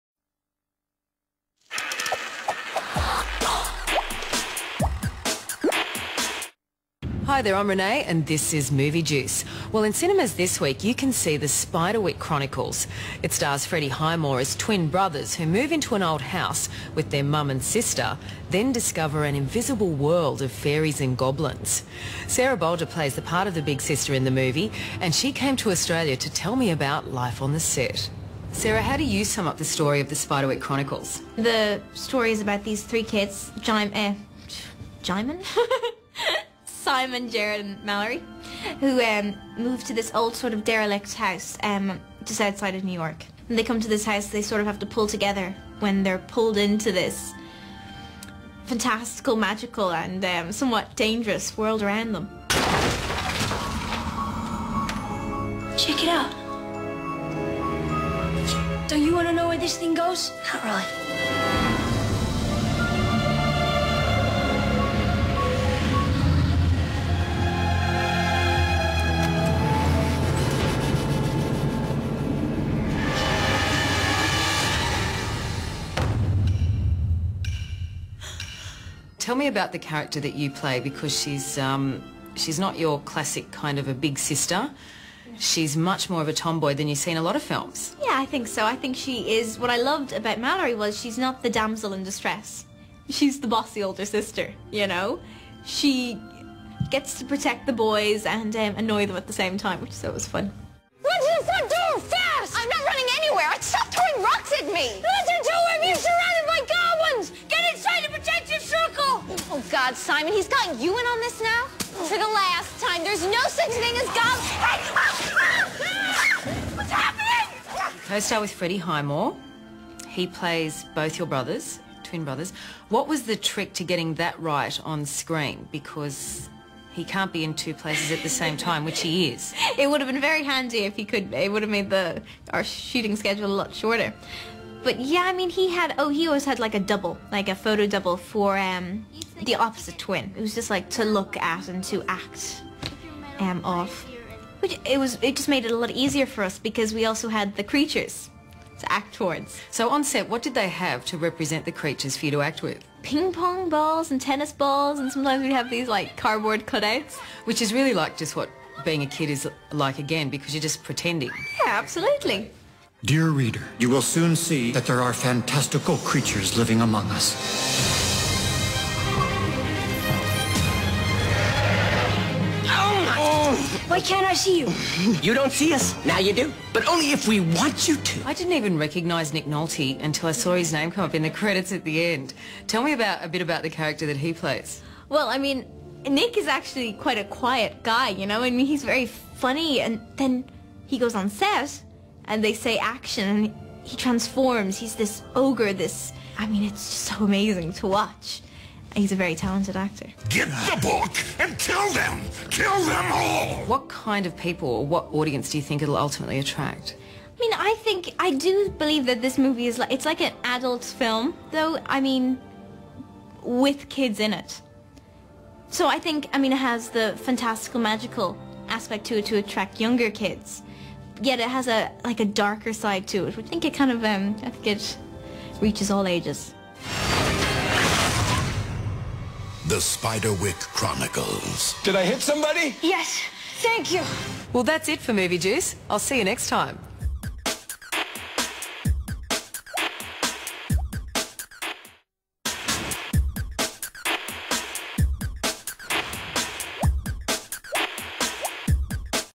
Movie Juice: The Spiderwick Chronicles
Renee interviews Spiderwick Chronicles star Sarah Bolger about her role in the new hit movie.